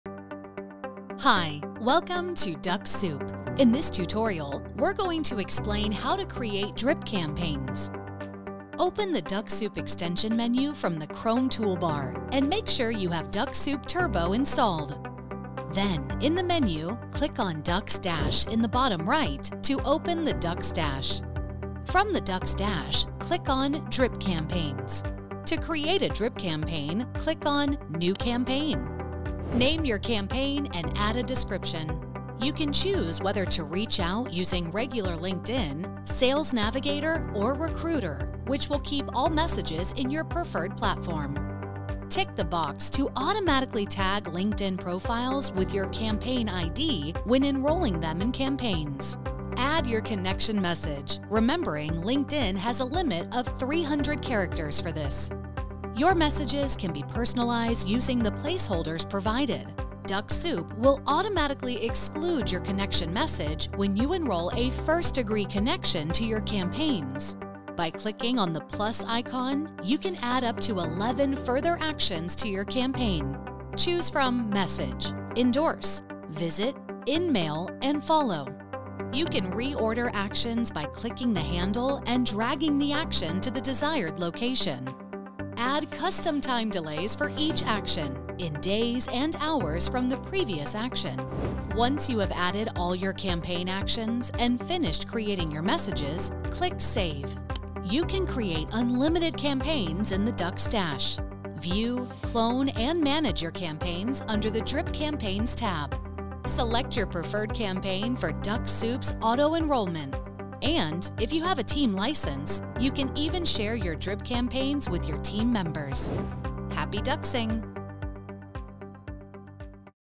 Dux-Soup Easy Tutorials - 13 - Creating Drip Campaigns
Learn how easy it is to create automated LinkedIn drip campaigns with Dux-Soup Turbo, available

Define unlimited sequences, each with up to 12 actions, selecting from Follow, InMail, Endorse, Connection request, and Direct message actions.

Set custom delays between actions and personalize your messages for better conversion rates.

In this video, you'll also discover how to share your LinkedIn campaigns with Team members, and collaborate on outreach whilst monitoring campaign results with ease.

Accelerate your business and scale your pipeline with Dux-Soup Turbo.